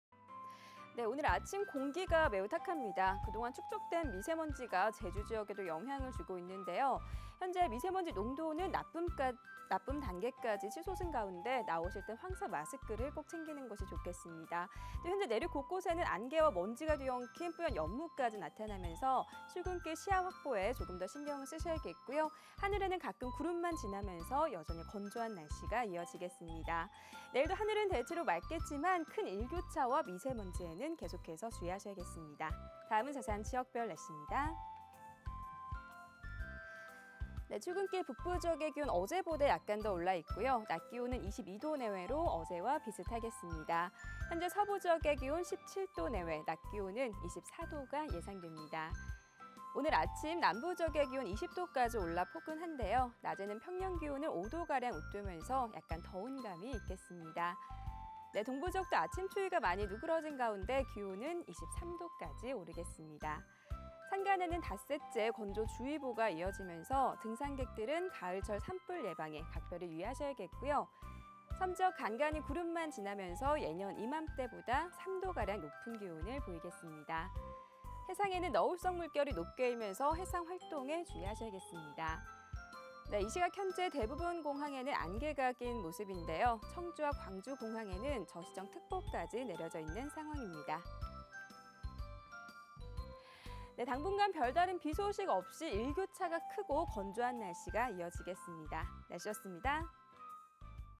기상정보 송연희
CG1.오늘
아침 출근길 공기가 매우 탁합니다.
그동안 축적된 미세먼지의 영향으로 제주도 미세먼지농도가 나쁨 단계를 보이고 있는데요.
장시간 야외활동은 되도록 자제하시고 마스크도 꼭 챙기는 것이 좋겠습니다.
현재 내륙 곳곳에는 먼지와 안개가 뒤엉킨 뿌연 연무가 낀 곳이 있고요.
하늘에는 가끔 구름만 지나면서 여전히 건조한 날씨가 이어지겠습니다.

CG2.내일
내일도 대체로 맑은 가운데 활동하기 좋겠지만,
큰 일교차에는 주의하셔야겠습니다.

(북부)
출근길 북부지역의 기온 어제보다 약간 더 올라 있고요.
낮 기온은 22도 내외로 어제와 비슷하겠습니다.

(서부)
현재 서부지역의 기온 17도 내외,
낮 기온은 24도가 예상됩니다.

(남부)
오늘 아침 남부지역의 기온 20도까지 올라 포근한데요.
낮에도 평년 기온을 5도 가량 웃돌면서 조금 더운 감이 있겠습니다.

(동부)
동부지역도 아침 추위가 많이 누그러진 모습입니다.

(산간)
산간에는 닷새째 건조주의보가 이어지면서
등산객들은 가을철 산불예방에 각별히 유의하셔야겠고요.

(섬 지역)
섬지역 간간이 구름만 지나면서 예년 이맘 때 보다 3도가량 높은 기온을 보이겠습니다.

(해상)
해상에는 너울성 물결이 높게 일면서 해상활동에 주의하셔야겠습니다.

(공항 날씨)
이 시각 현재 대부분 공항에는 안개가 낀 모습이고요.
청주와 광주에는 저시정특보가 내려져 있는 상황입니다.

(주간 날씨)
당분간 별다른 비 소식 없이 건조한 날씨가 이어질 전망입니다.